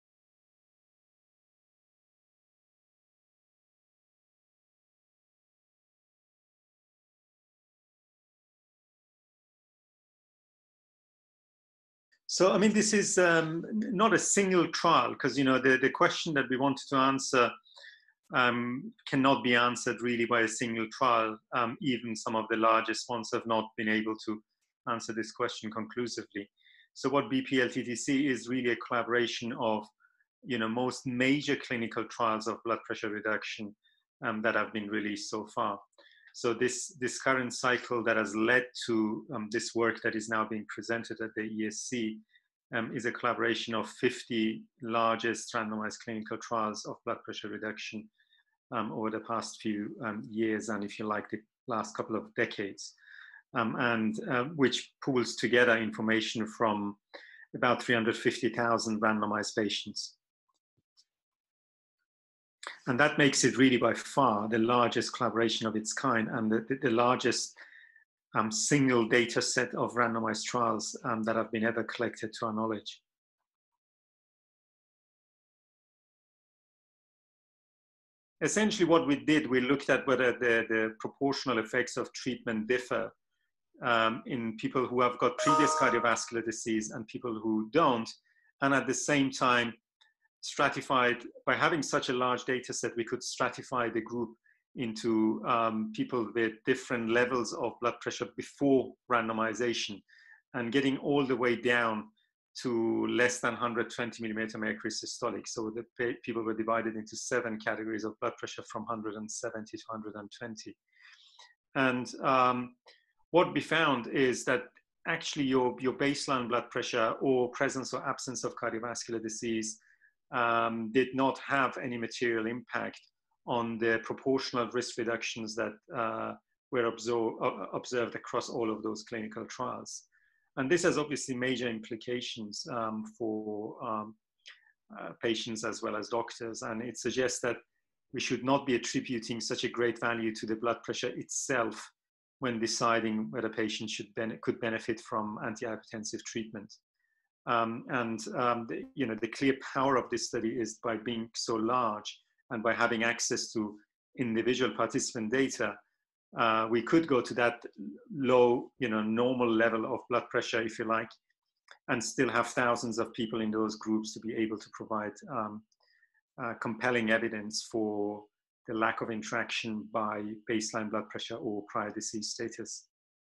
Kazem Rahimi, MD: BPLTTC Results
Kazem Rahimi, MD, offers further insight into the design and results of the BPLTTC trial presented at ESC Congress 2020.